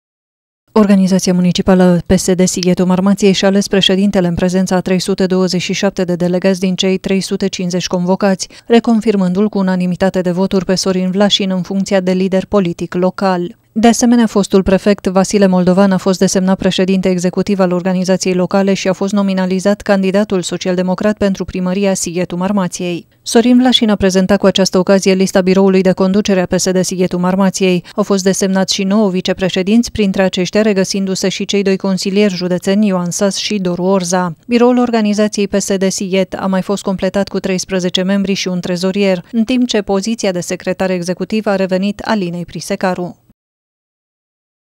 Sorin Vlașin, reconfirmat președinte al PSD Sighet
Sorin Vlașin a fost reconfirmat președinte al PSD Sighetu Marmației. Acesta a fost ales de 327 dintre cei 350 delegați care au fost convocați.